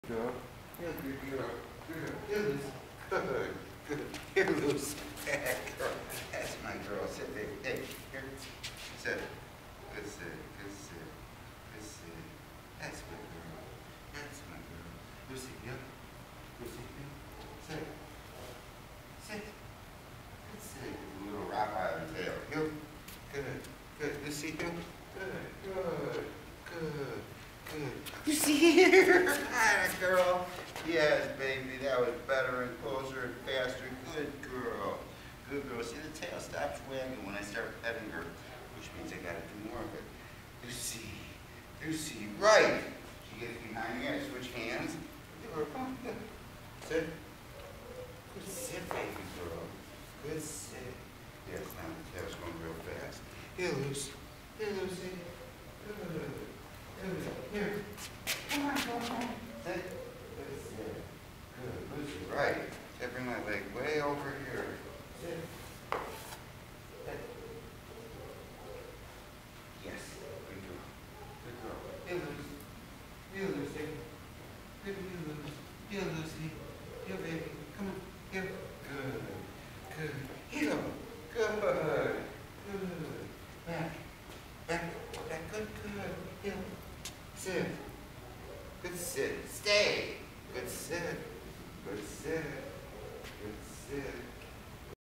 Allan Ross Dog Training
Lucy B. is a Beagle mix in her second week of Board and Train.  Here we practice obedience in the Canine College.  She is reactive to distractions and by following the leader can relax.